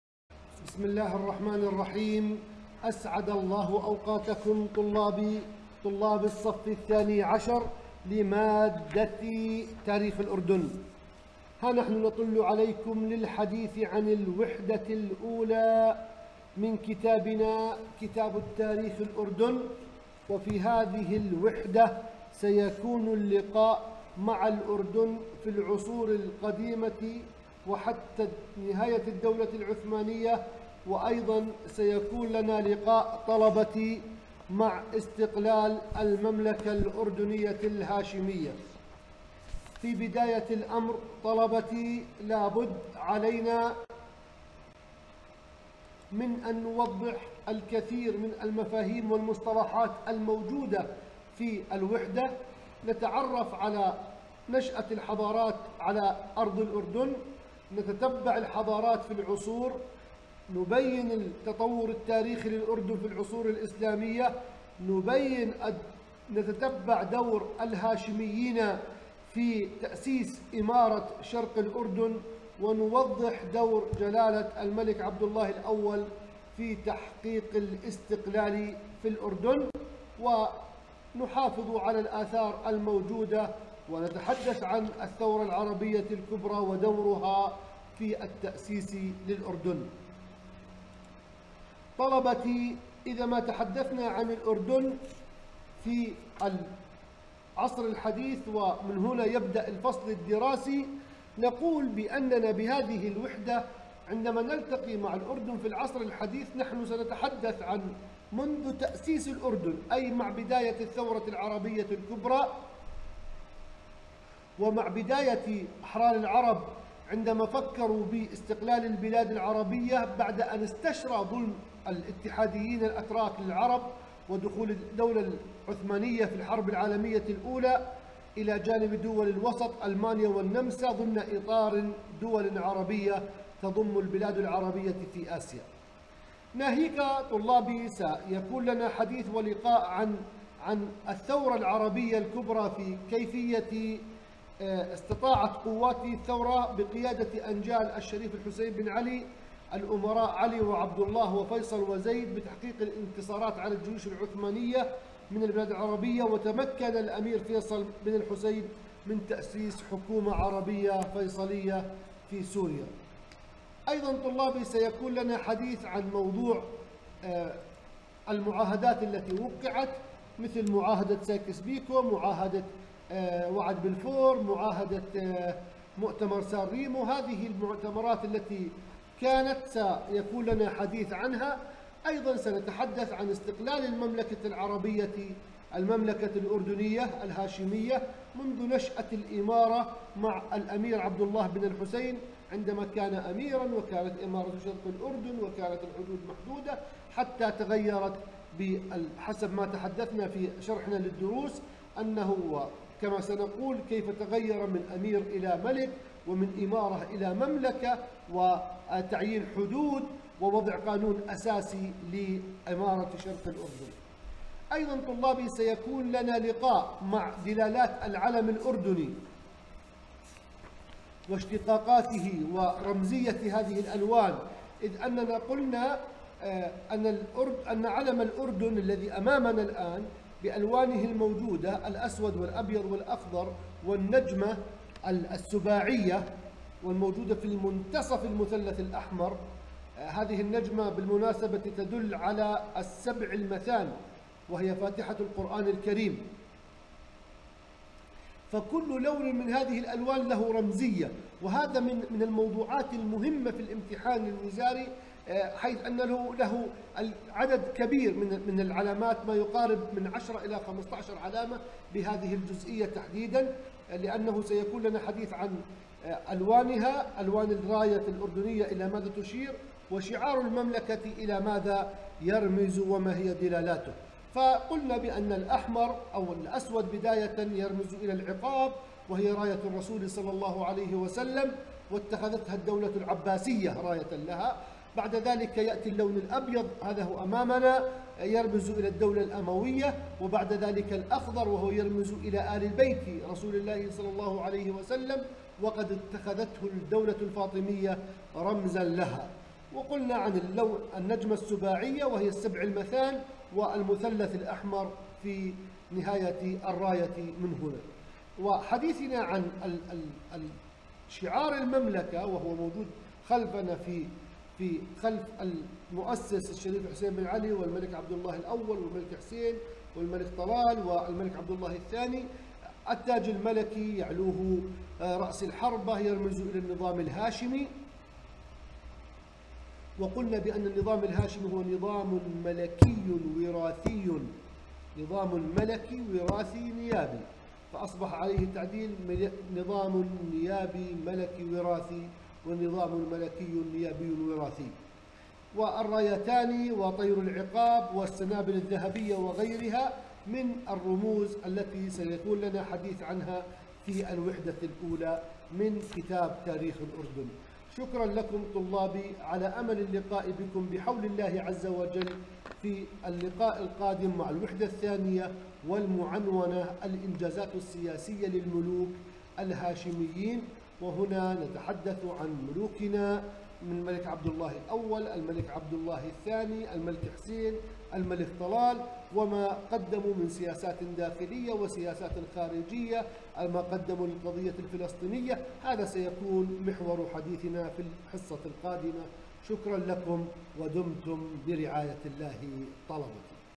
فيديو الوحدة الأولى الأردن في التأسيس والاستقلال صف 12- الاستاذ منيزل
يتحدث هذا الفيديو عن الوحدة الأولى من كتاب تاريخ الأردن متحدثا عن أهم الموضوعات التي سيتم دراستها خلال دراستنا للوحدة وعن أهم النقاط المتعلقة في الوحدة .